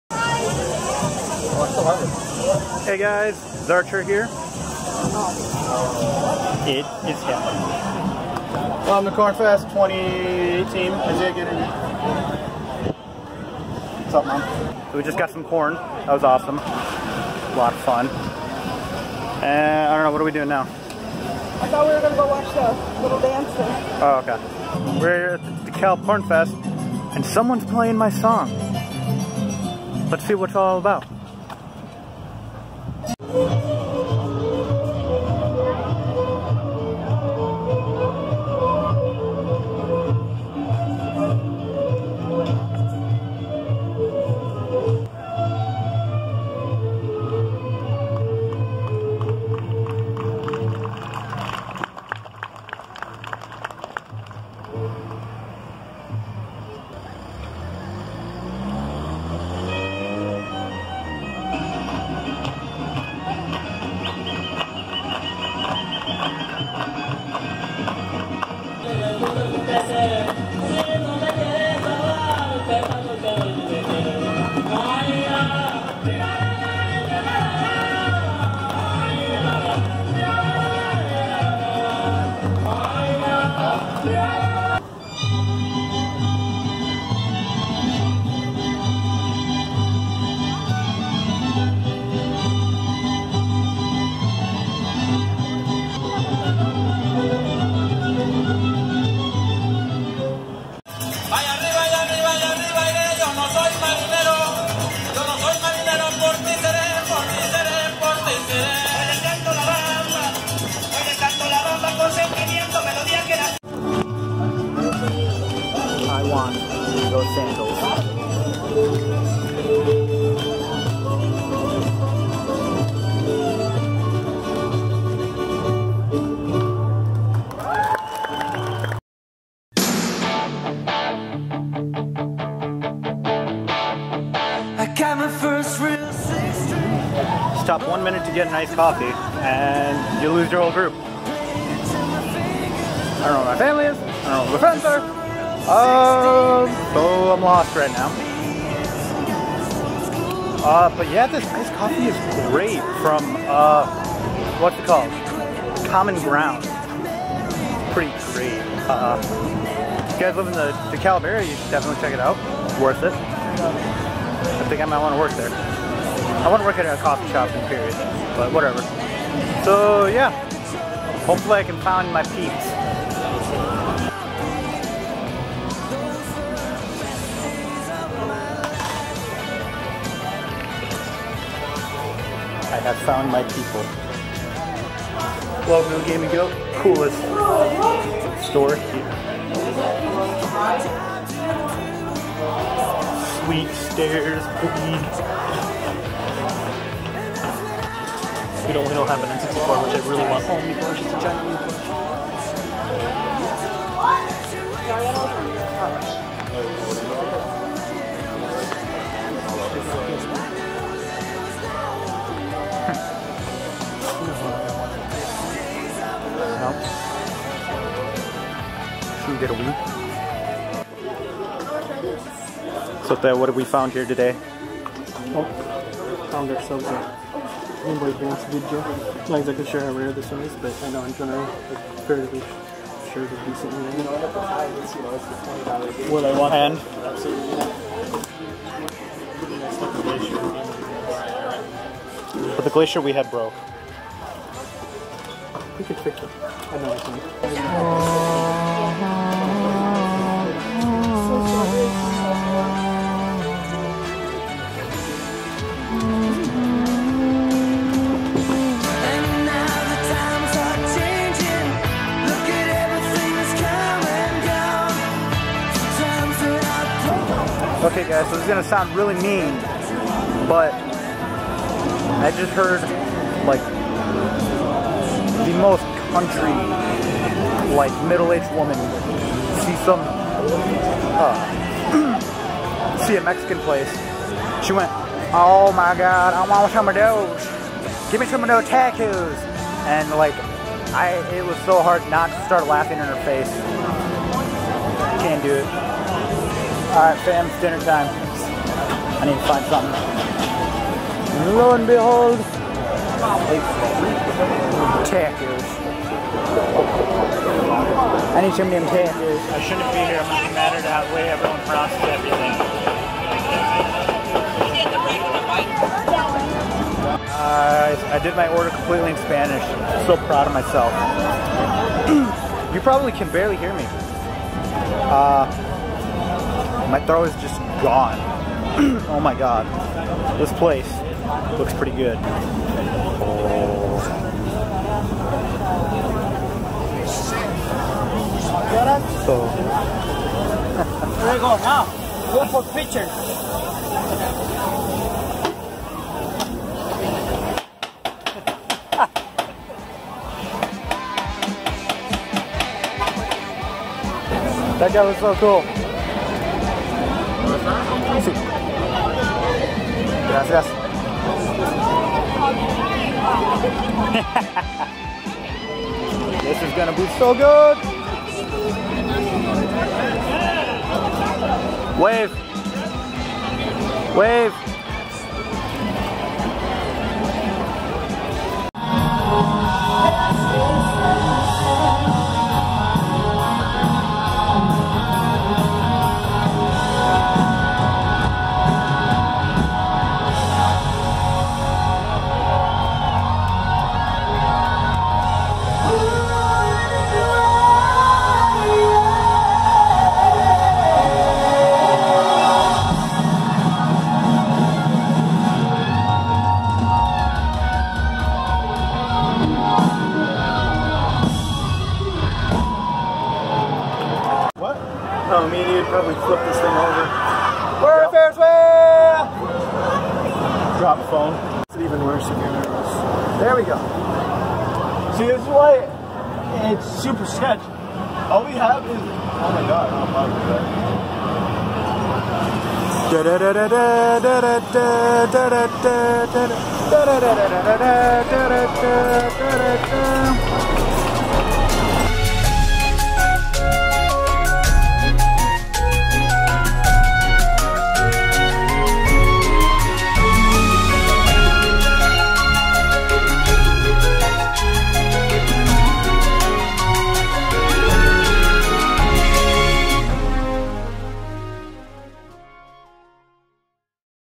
My Last Summer Vacation - DeKalb Cornfest 2018 ||Zar Dominguez
Last weekend was the DeKalb Cornfest. Here's a vlog. I only went on Saturday because I had school on Friday and my meeting on Sunday. Either way, it was a lot of fun and thought it would provide a little break from the "Dear Highschool..." Project. I hope you enjoy

SOCIALS-